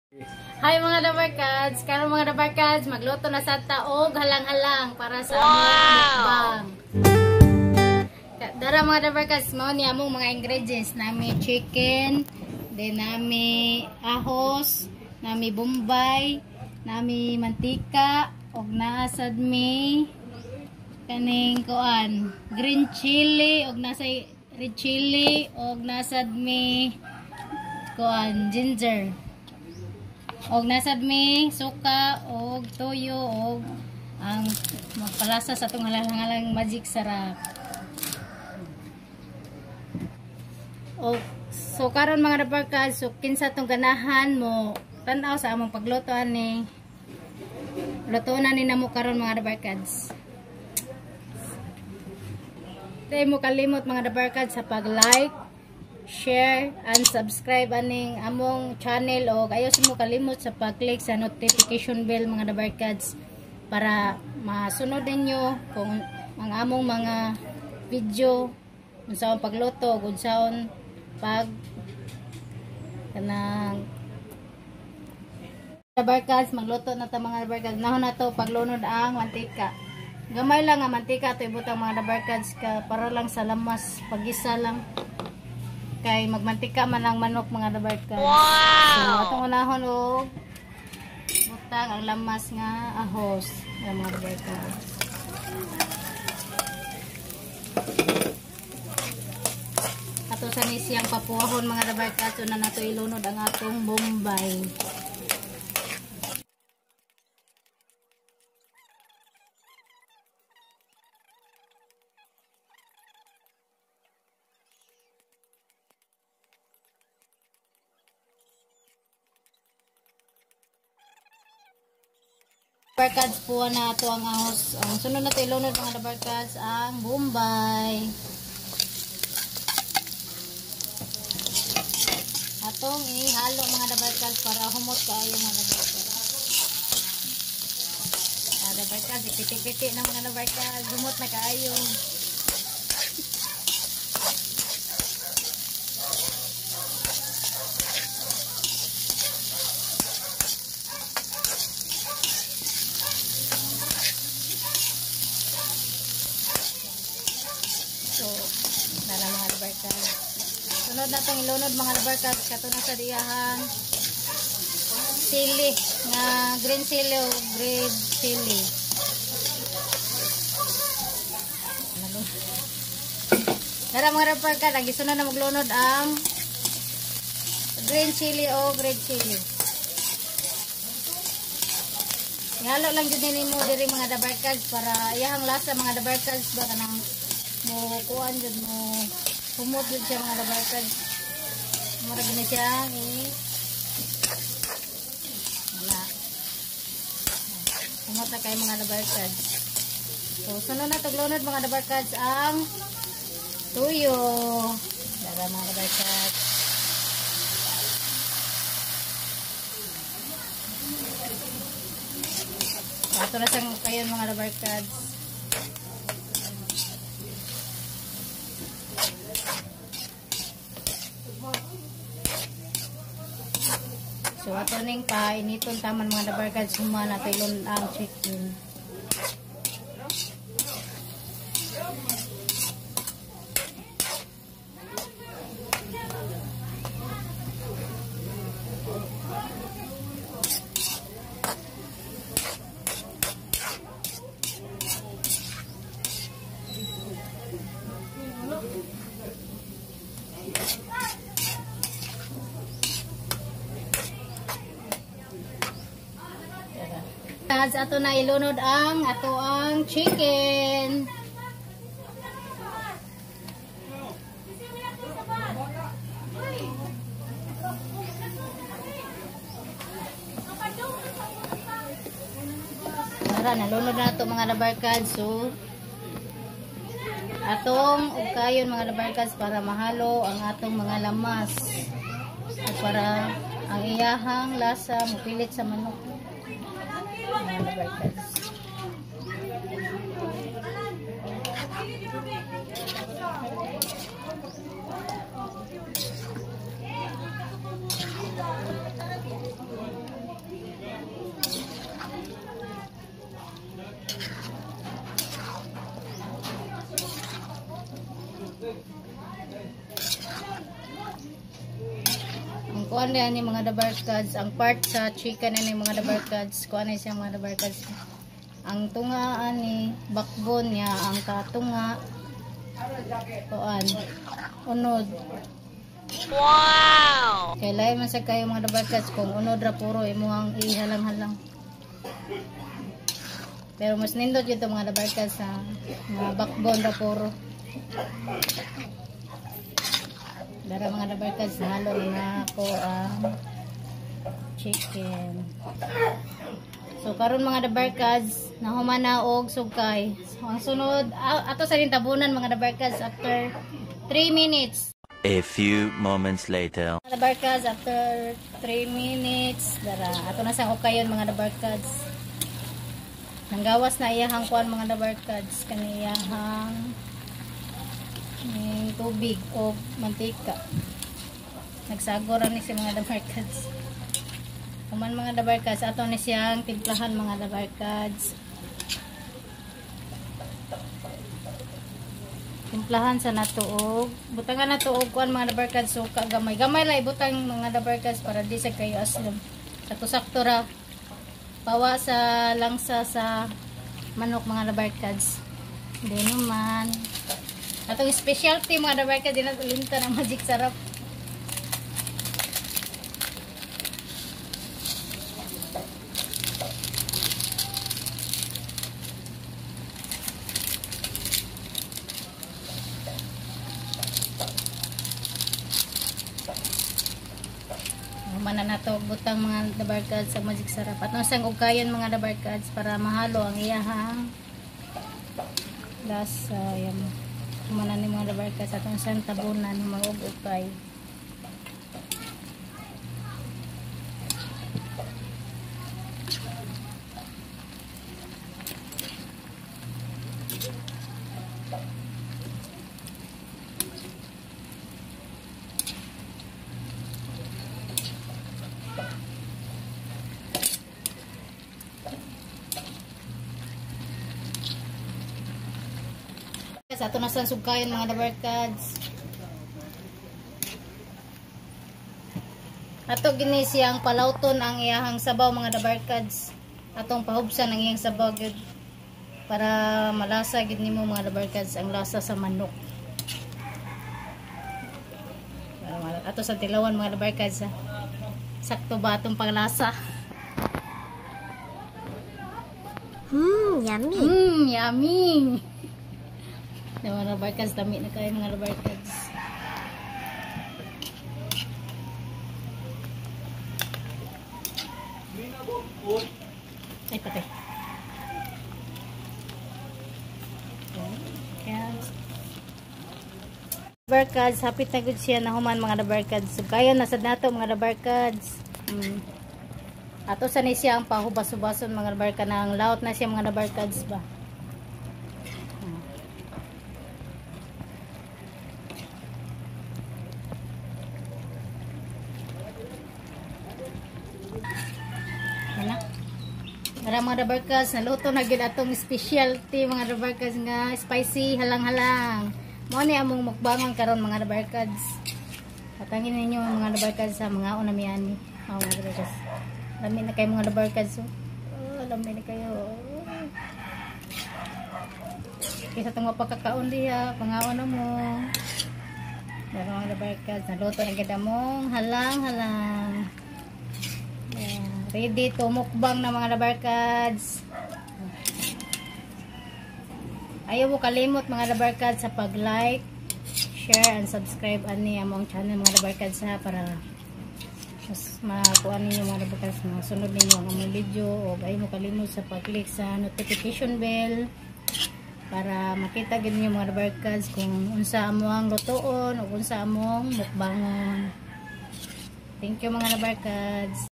HOW TO COOK SPICY HALANG HALANG NA MANOK with Dabarkads Netzkie
Eng.
1 1/2 kilo chicken backbone part
1/4 cup oil
1/2 cup soy souce
1/2 cup vinegar
1 glove garlic
1 glove onions
200g ginger
Seasonings
Acording to taste